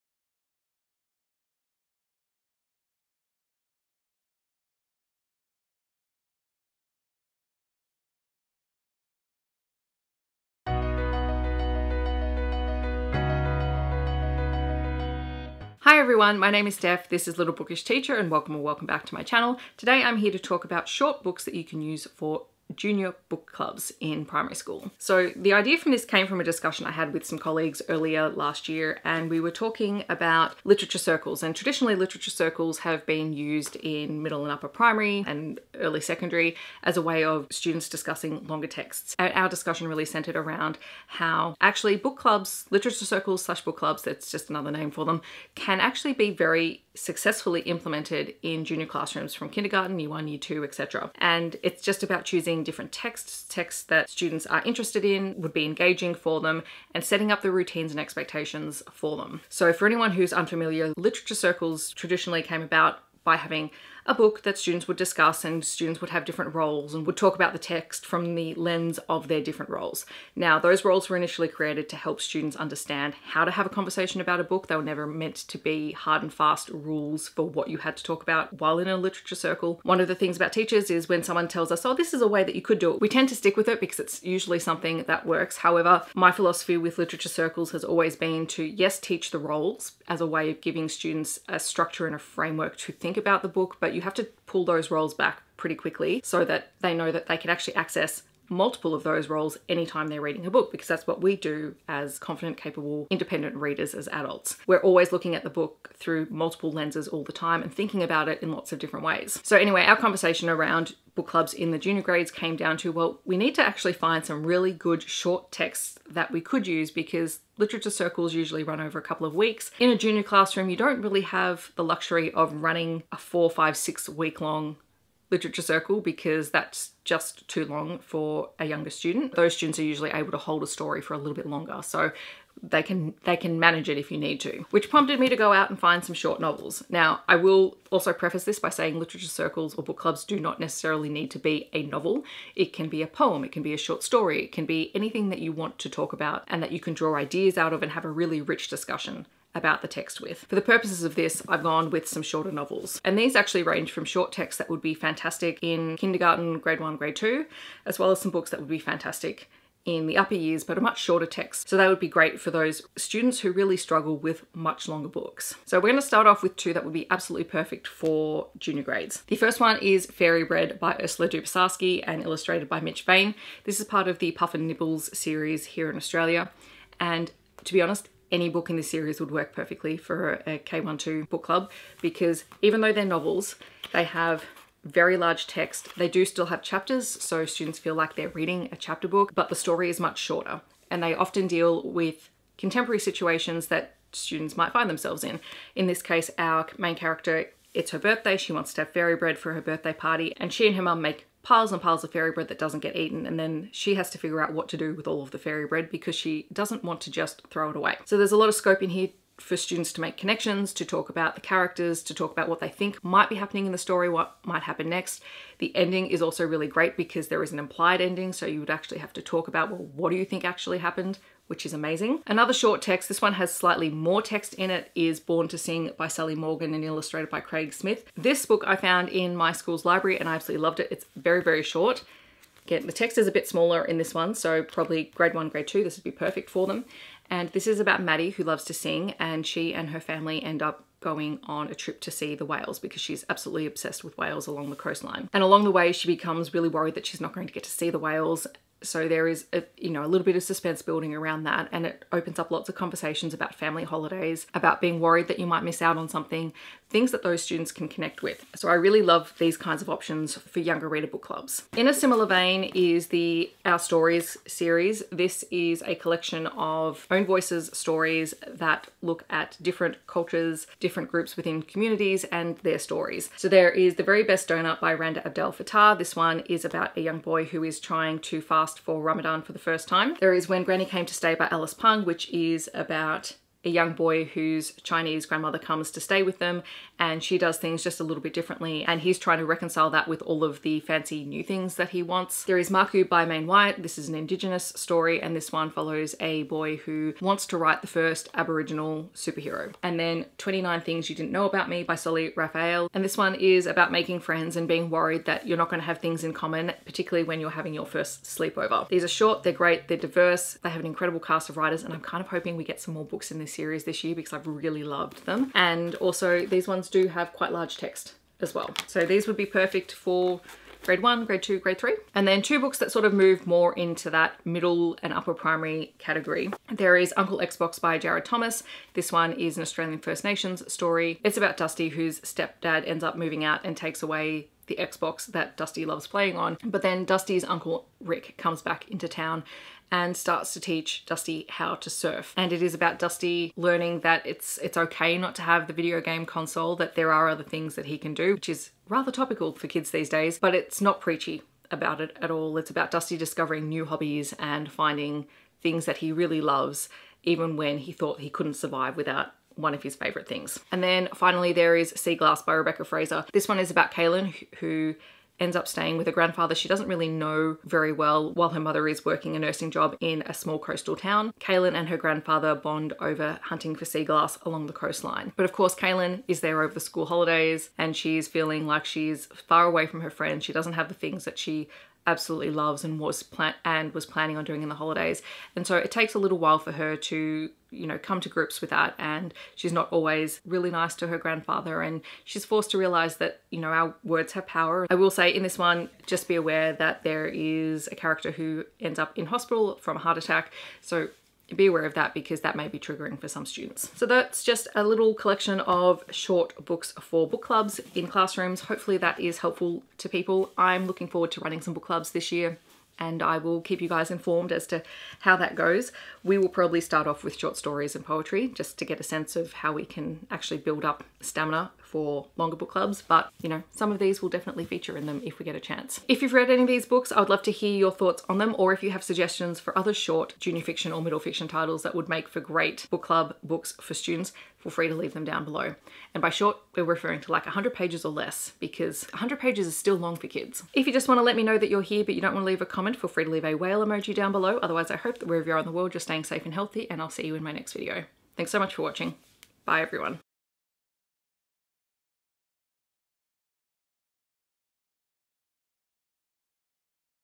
Short Books For Kids' Book Clubs
Today I’m sharing a range of books that are all about 100 pages or less for book clubs in primary school classrooms. Shorter texts allows for more focused (and shorter) book club - or literature circle - discussions, and also gives teachers the opportunity to extend younger readers into book club discussions.

Books Mentioned
Fairy Bread - Ursula Dubosarsky
Born to Sing - Sally Morgan
The Very Best Doughnut - Randa Abdel-Fattah
When Granny Came to Stay - Alice Pung
Maku - Meyne Wyatt
29 Things You Didn’t Know About Me - Solli Raphael
Uncle X-Box - Jared Thomas
Sea Glass - Rebecca Fraser

Timestamp
0:00 Acknowledgement of Country
0:16 Intro
2:30 Short books for Book Clubs
6:43 Wrap-Up